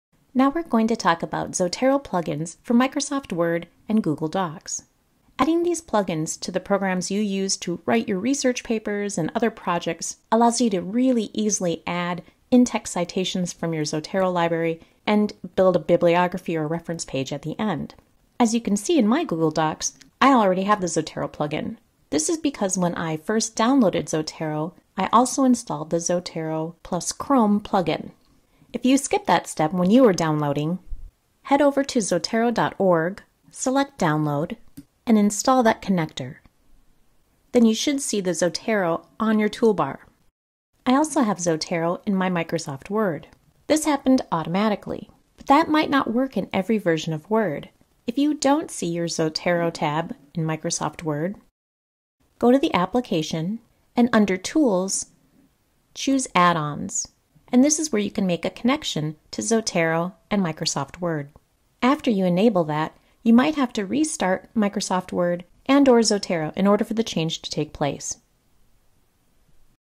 Zotero Plugins for Word and Google Docs
Use Zotero plugins for Google Docs and MS Word.

This video was made on behalf of the Western Washington university Libraries for the Hacheral Research and Writing Studio